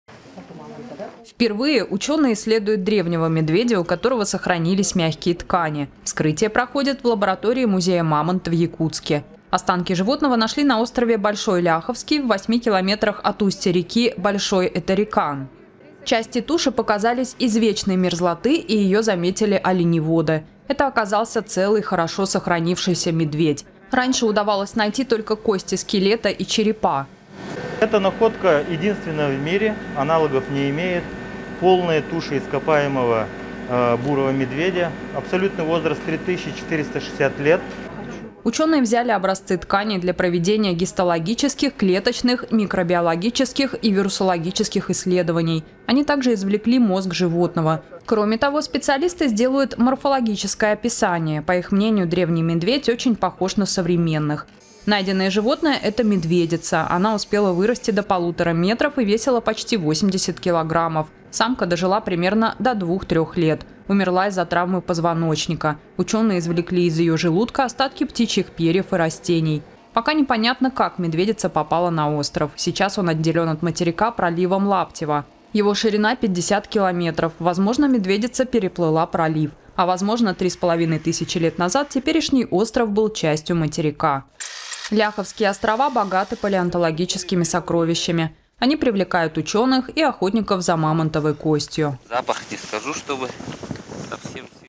Учёные впервые исследуют древнего медведя с сохранившимися мягкими тканями
В Якутске исследуют останки древнего медведя с сохранившимися мягкими тканями. Такая возможность появилась у учёных впервые. Раньше находили только кости и черепа.